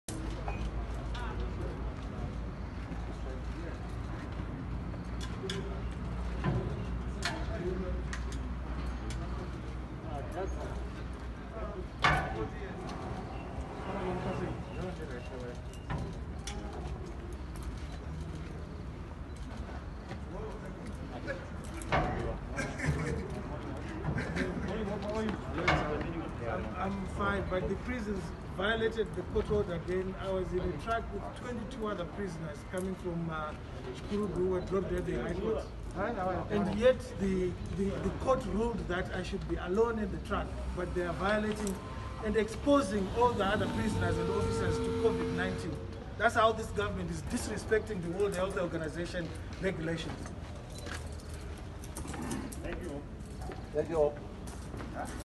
VaJob Sikhala naHopewell Vachisvika Padare Nhasi
Dare ramejasitiriti rinorasa chikumbiro chaVaJob Sikhala chekuti vasaiswe pa remand, mutongo waHopewell Chin’ono uchitarisirwa mangwana uye waMuzvare Mahere nemusi weChishanu panyaya imweyo. Vhidhiyo ina Thomas Chiripasi.